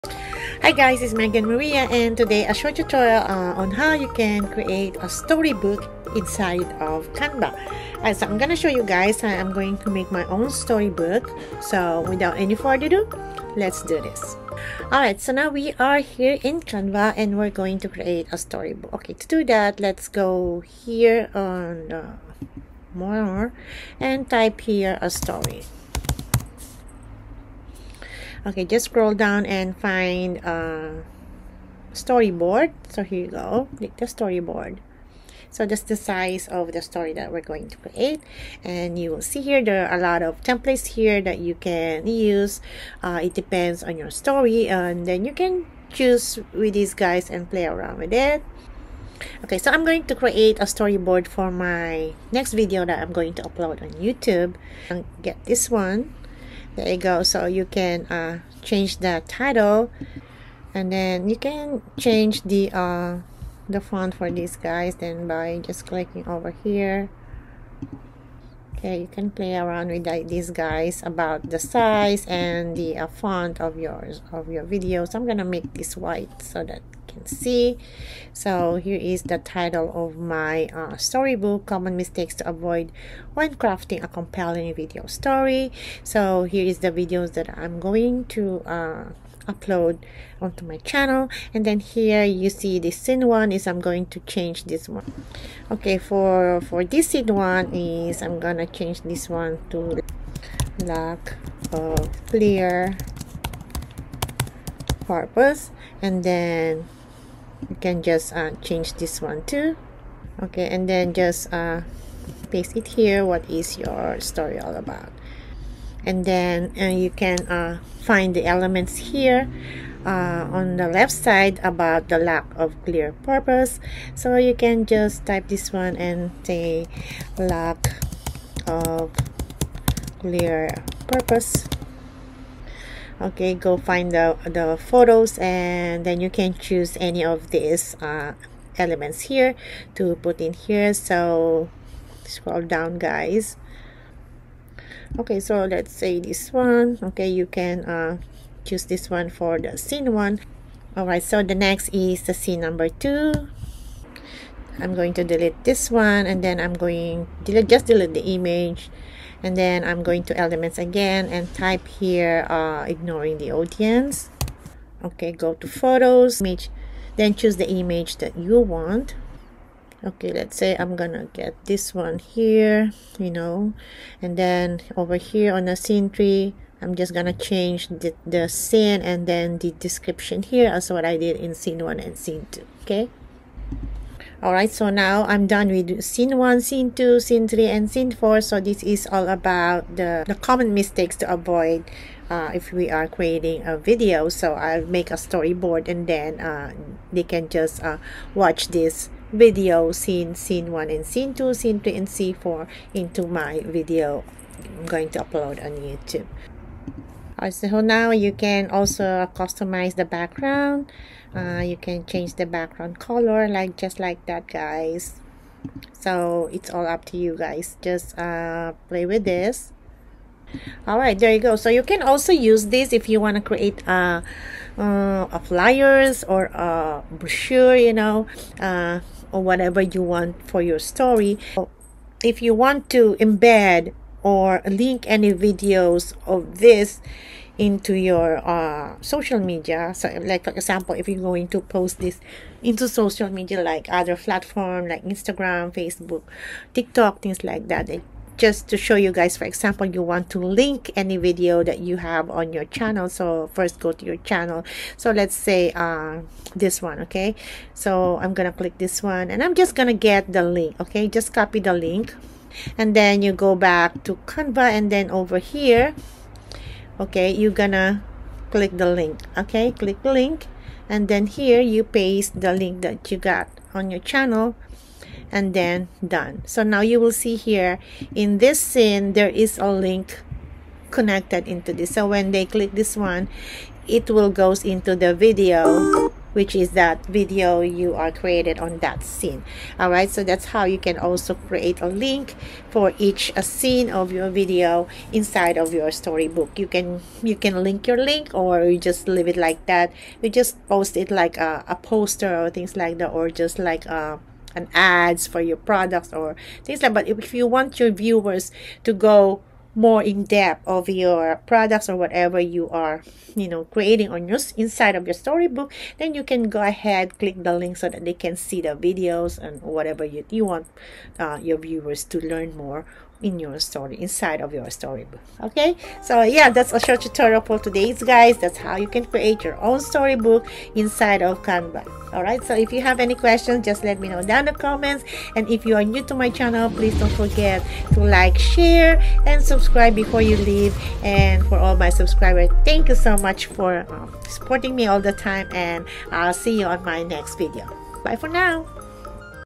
How to Create a Stunning Storyboard in Canva-Step-by-Step Guide
✨ Want to create a professional-looking storybook without any design experience? In this video, I'll show you how to make a stunning storybook in Canva step by step! Whether you're a writer, teacher, or aspiring author, this easy tutorial will help you design, format, and even publish your own book with Canva's powerful tools. Perfect for children's books, digital storybooks, and self-publishing!
Grow your channel: Sign up: Best AI tools for Youtubers